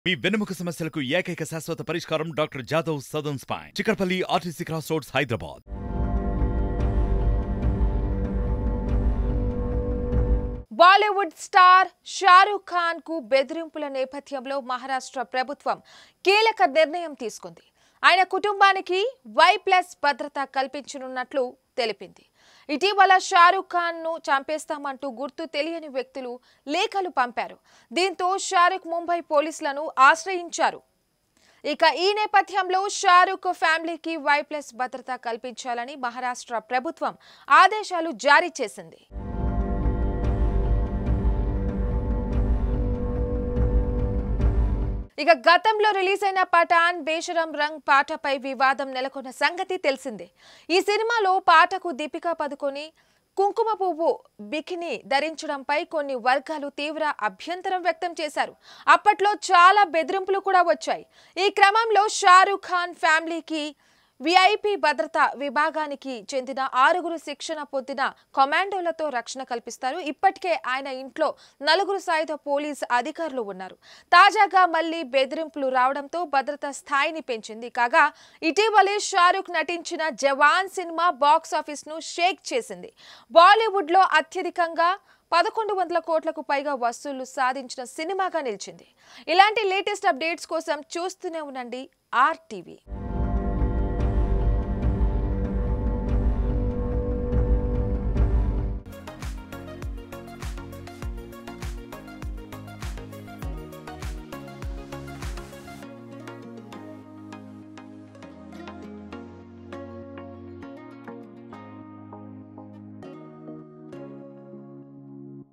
Shah Rukh Khan Receives Threat Calls | షారుఖాన్ కు బెదిరింపు కాల్స్ | Jawan Effect | RTV Live
Shah Rukh Khan Receives Threat Calls | షారుఖాన్ కు బెదిరింపు కాల్స్ | Jawan Effect | RTV LiveFor More News Updates,